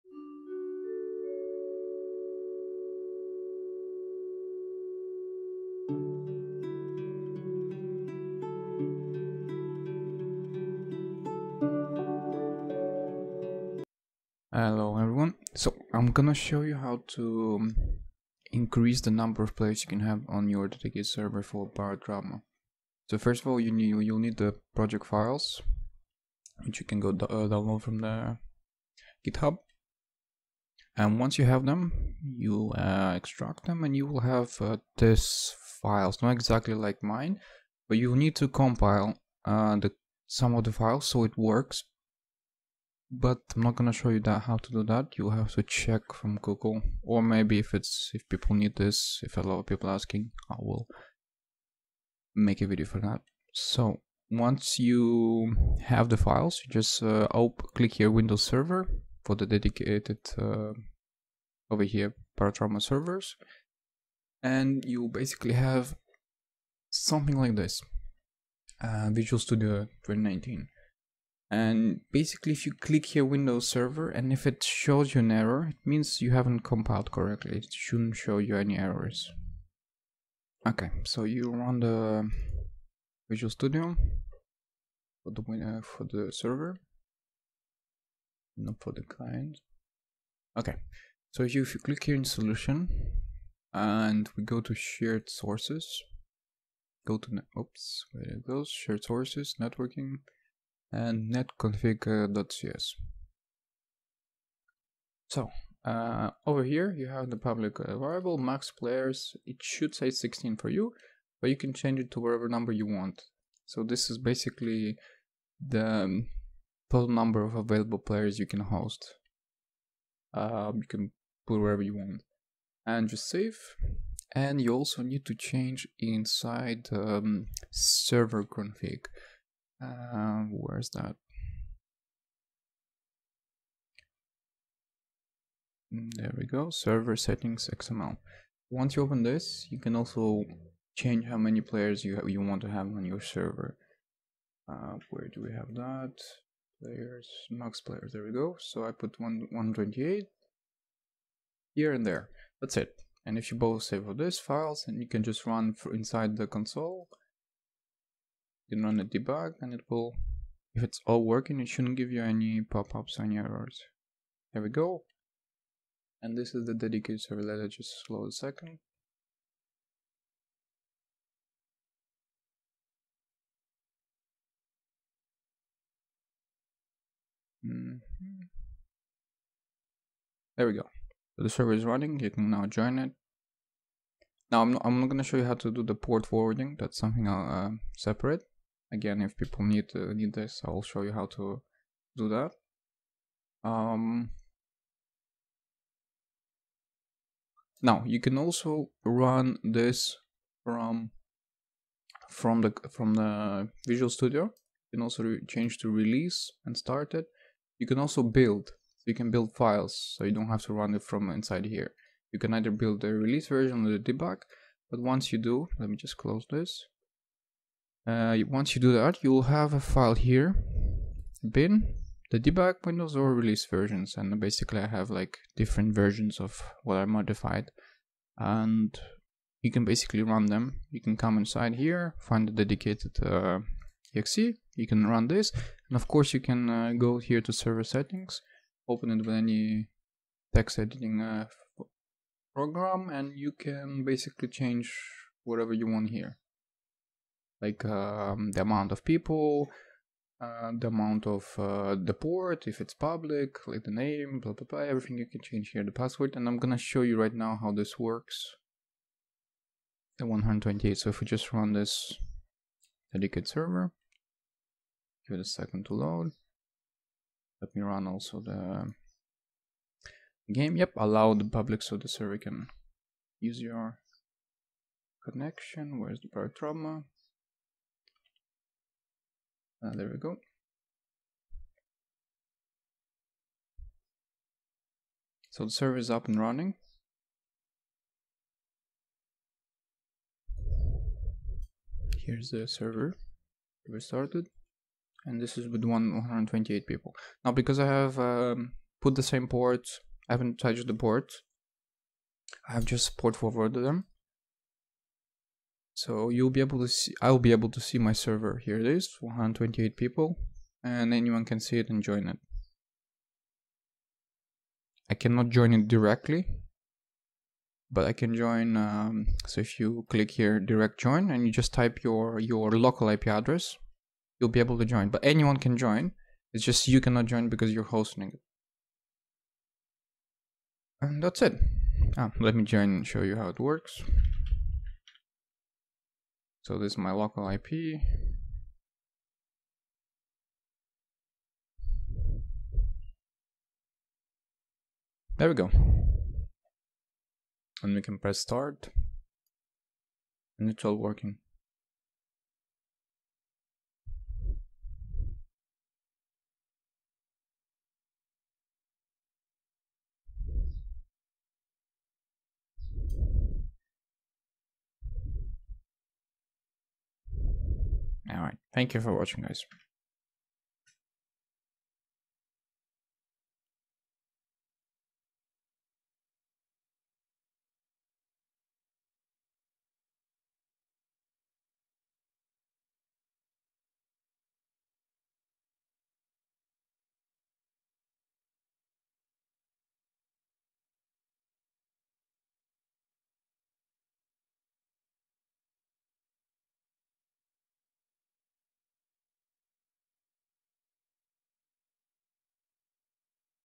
How to Increase MaxPlayers in Barotrauma (Dedicated Server)
How to Increase Max-Players in Barotrauma (Dedicated Server)
Advanced
1. Compile the game files from github
2. Open Barotrauma-master\Barotrauma\BarotraumaServer\WindowsServer.csproj
3. Open \SharedSource\Networking\NetConfig.cs
4. Change MaxPlayers = 16; (This is to increase the max players you can have in your server.)
5. serversettings.xml (The total amount of maxplayers you want to have on the server. Can not go over the maxplayers in NetConfig.cs)